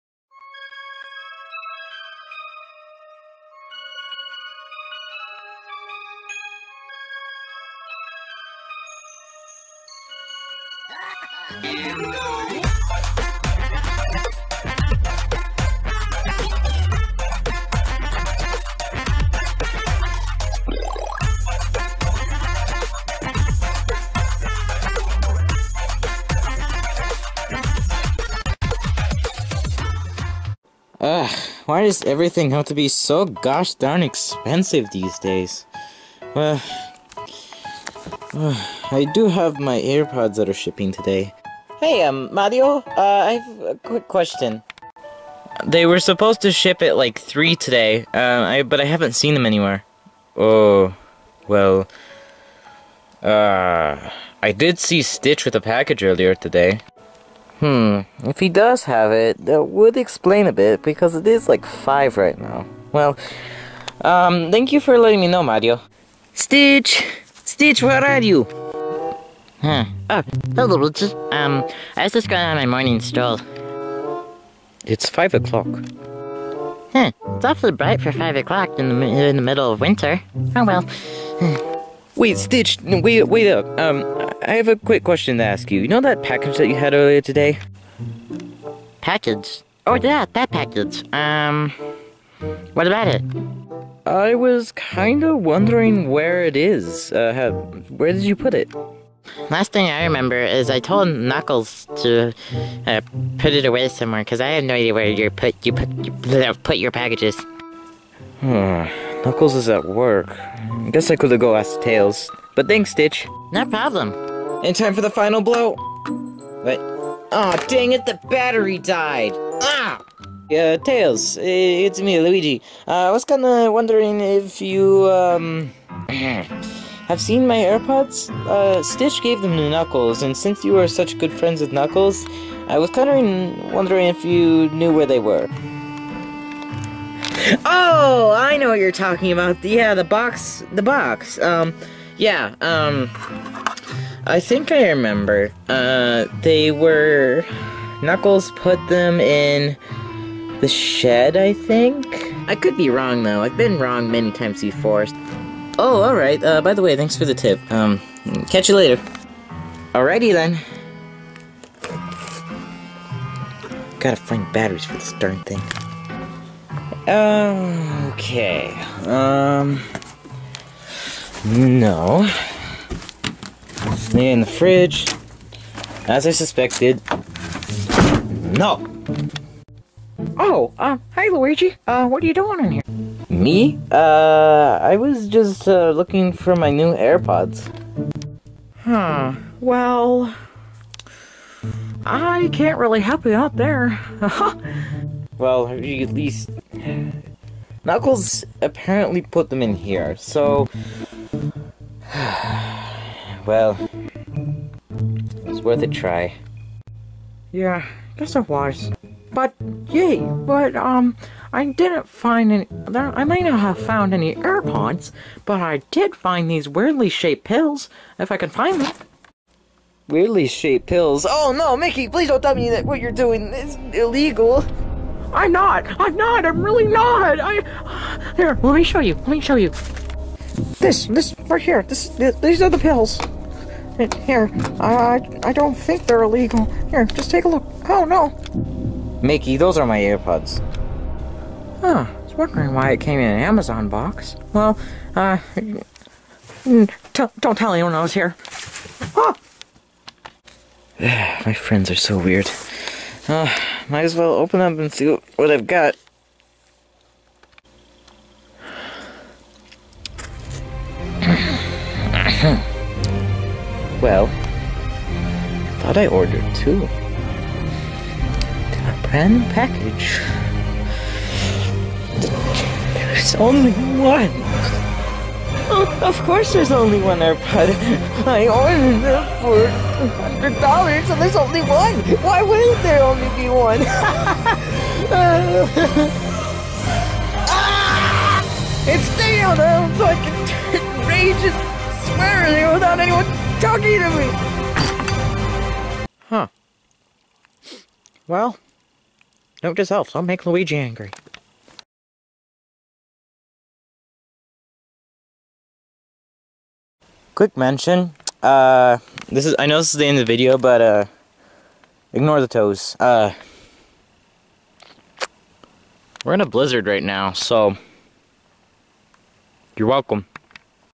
A Hunt for AirPods (Episode 19)
An expected package arrives but has gone missing.